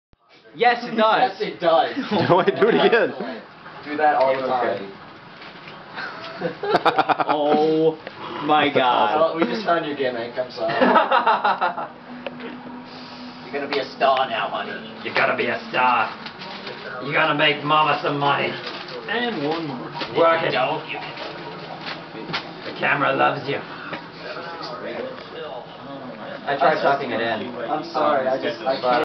AC 2009 - Hulex's New Suit
Hulex shows off his brand new fursuit to us for the first time, and less than an hour later, he's recorded by a local news crew, with Hulex eventually appearing on national news outlets.